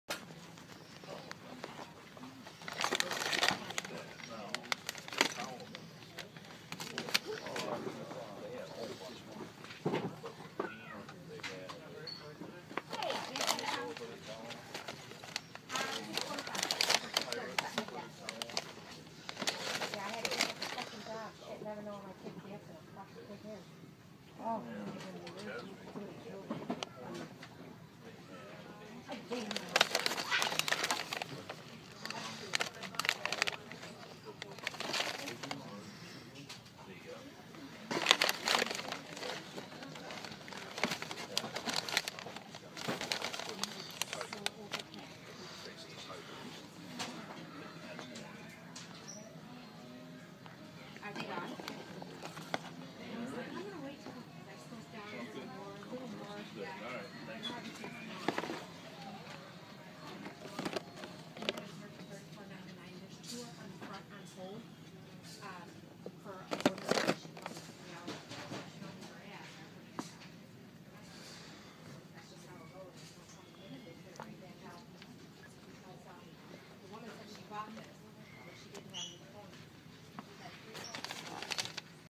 Halo mega bloks Series Delta found!!!
I went to Buffalo, then hit Target and this is what I saw.